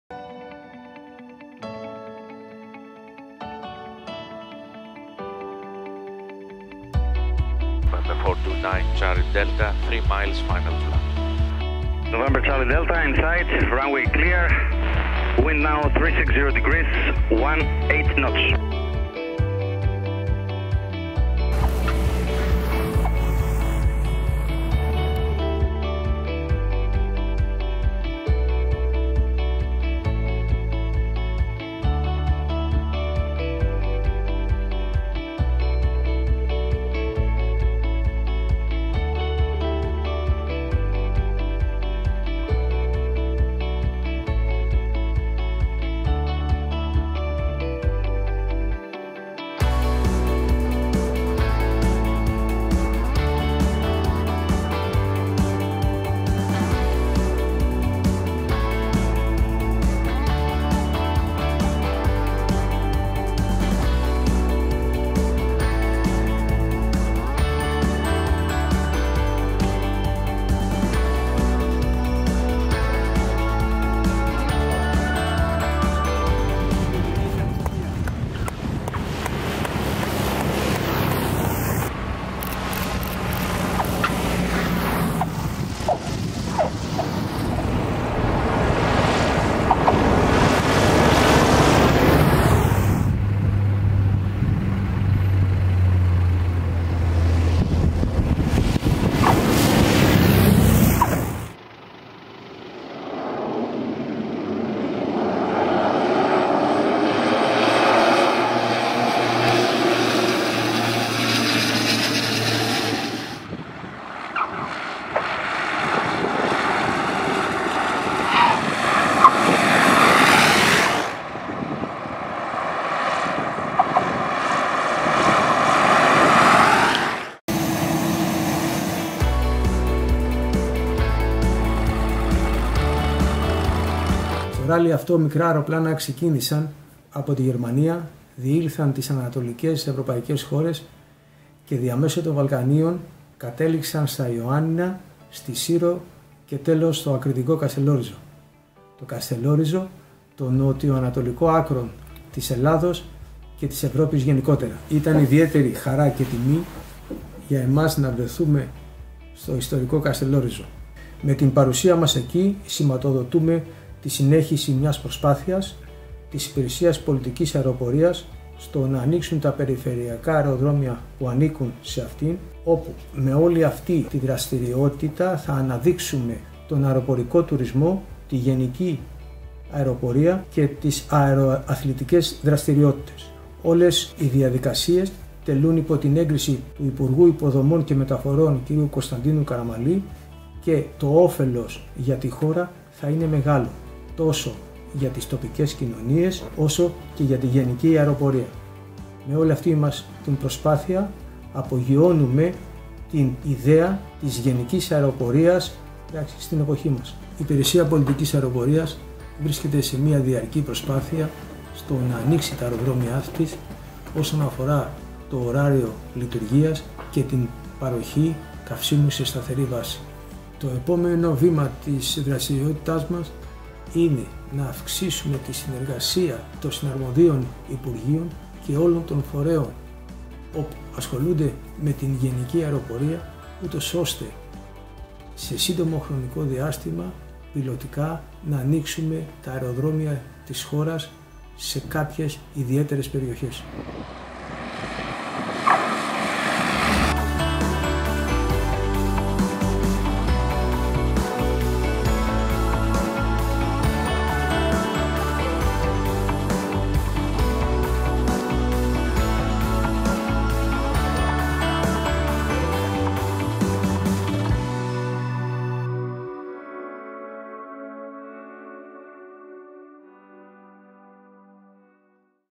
European Cirrus Rally - Passage through Greece_
The European Cirrus Rally 2021, has completed a successful passage through Greece. The event was facilitated by the Hellenic Pilots Association and co-organized by the German Pilotravel travel agency, as well as the Cirrus Aircraft Germany.

Eleven (11) private light aircraft, 8 of which were Cirrus SR22, arrived in Greece from central Europe, as part of an aviation rally which commenced in Berlin and flew all the way through to Kastelorizo island, the most southeastern part of Greece. Whilst in Greece, the visiting aircraft landed at the airport of Ioannina, in Syros island and in Kastellorizo island, while their crew, about 30 pilots and passengers stayed in luxurious hotels at each of the destinations.